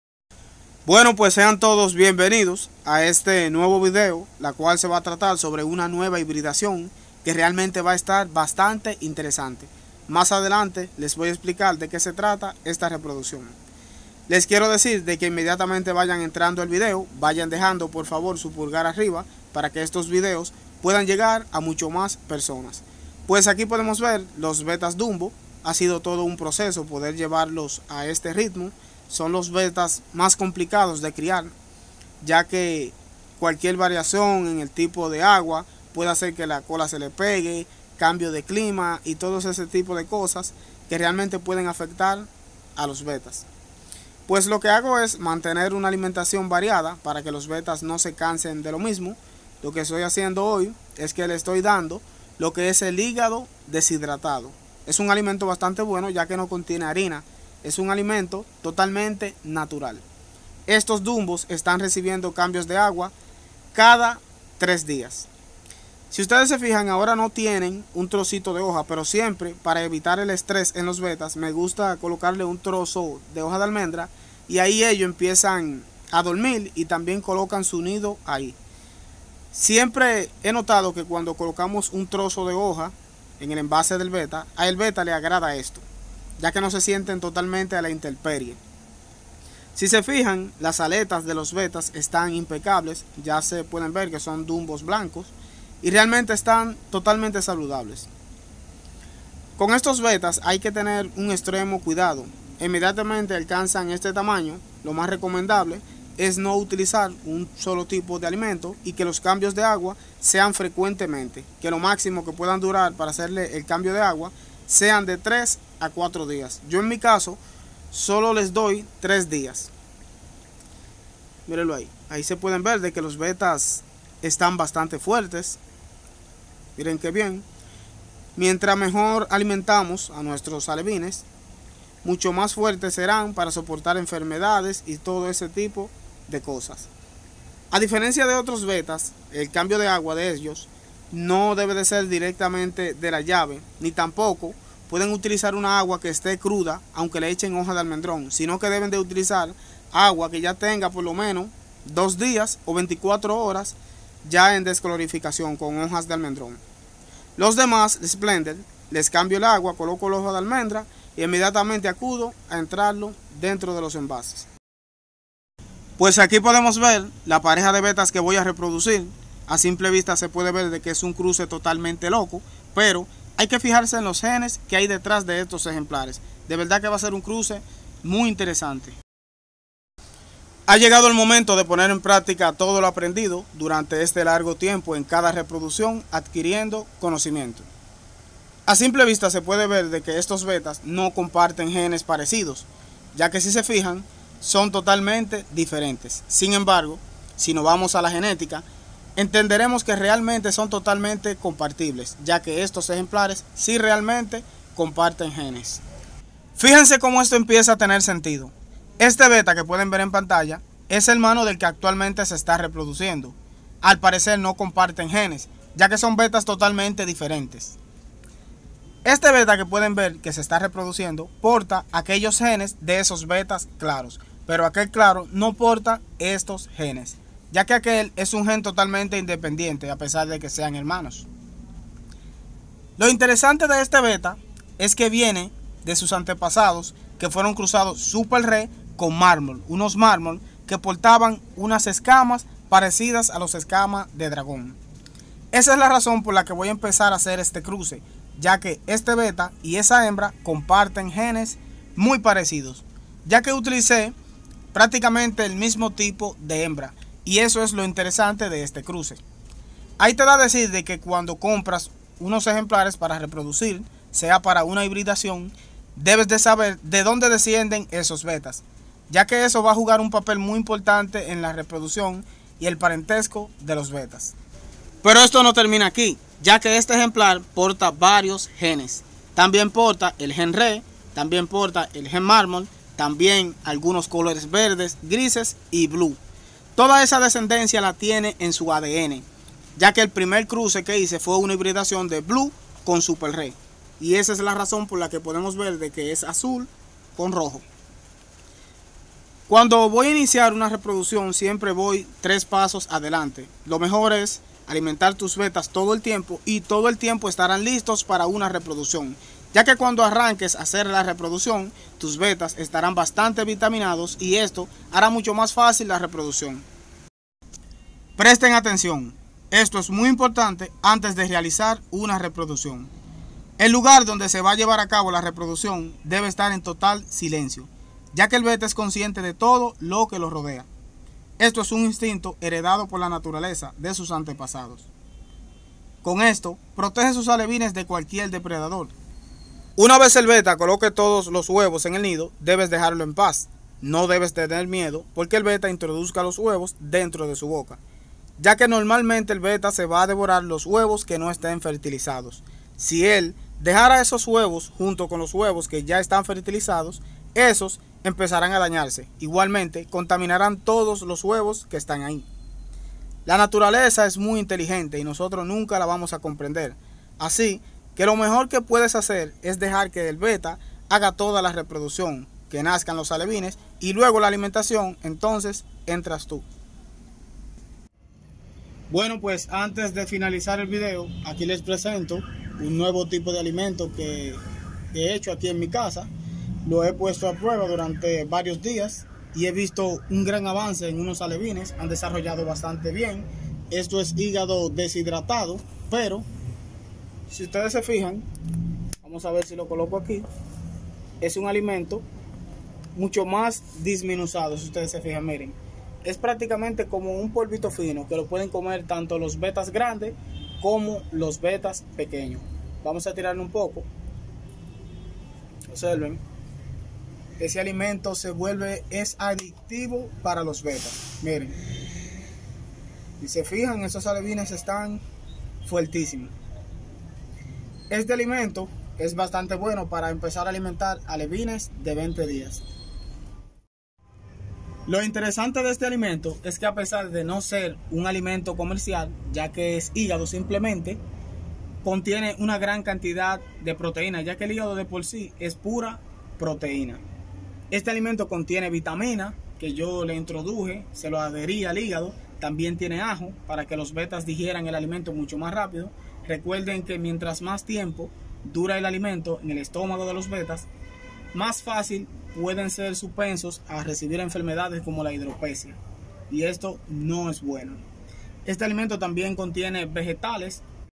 Reproducción de bettas esplenders paso a paso (como cuidar alevines de dumbos)
Saludos amigos en este video les muestro cómo reproducir bettas paso a paso y como cuidar alevines de bettas dumbos si el video te gustó no olvides suscribirte y activar la campanita 👍